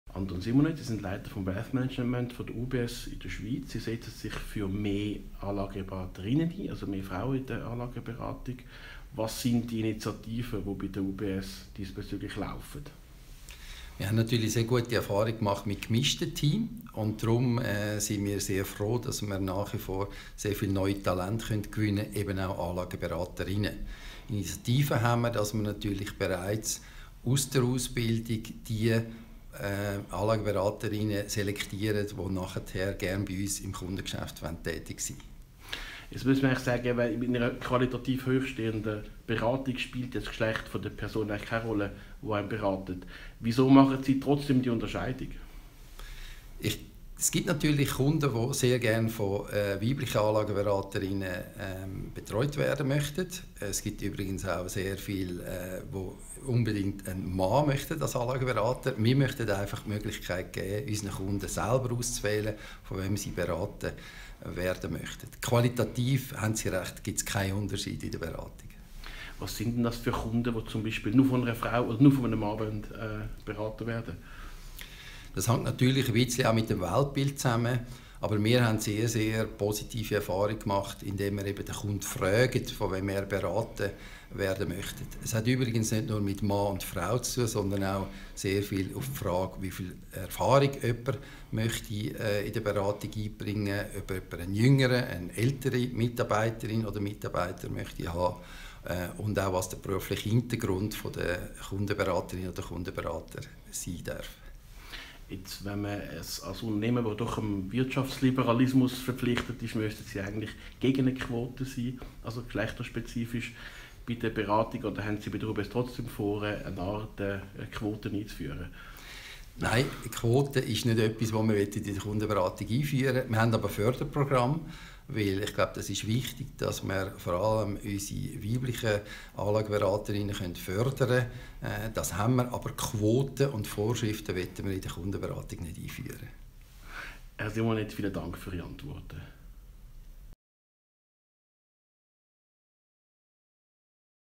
Anton Simonet, Leiter Wealth Management Schweiz UBS
Anlagekunden sind gut informiert, wie Anton Simonet feststellt. Bezüglich Börsenturbulenzen nimmt der Privatbankchef Schweiz der UBS grosse Gelassenheit wahr. Dafür werde die Digitalisierung nur zögerlich aufgenommen.